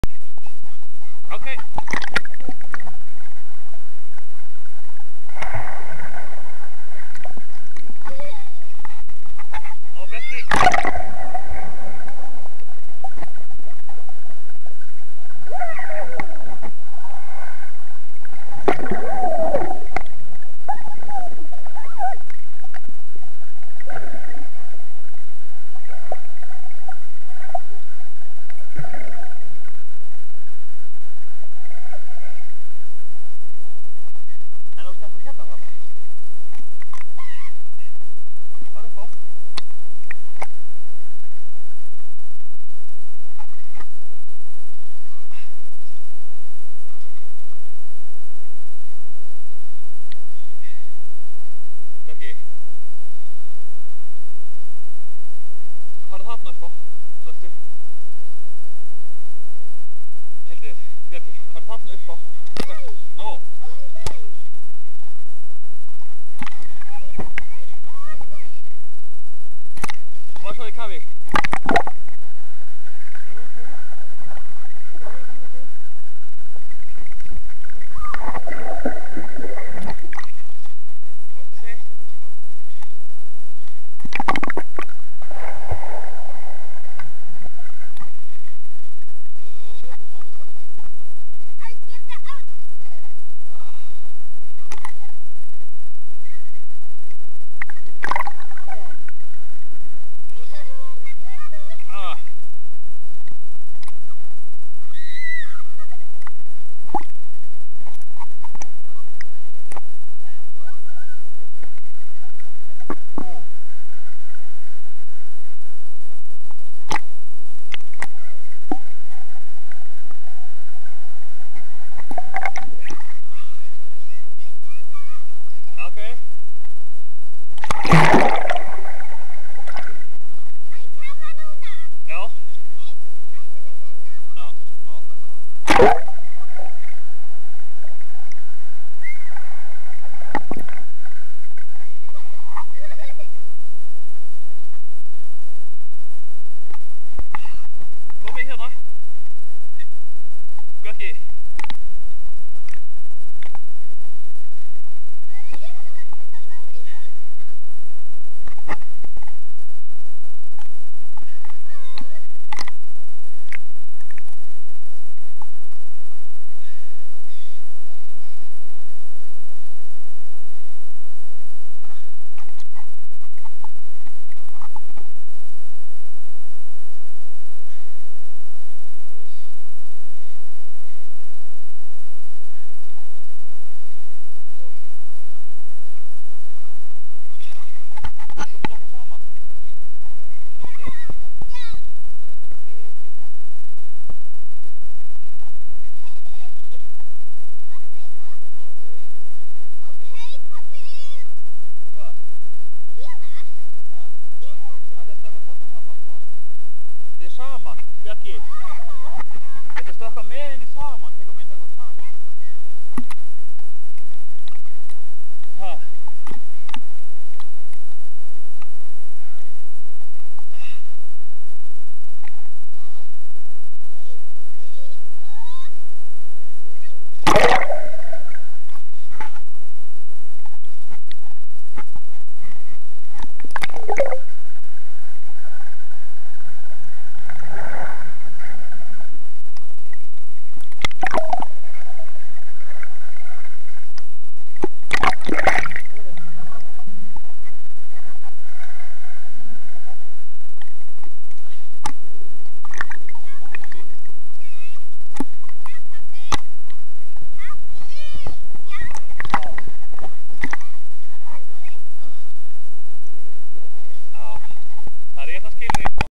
Sundferð 2014
Bjarki Freyr og Hildur Inga í sundi á Dalvík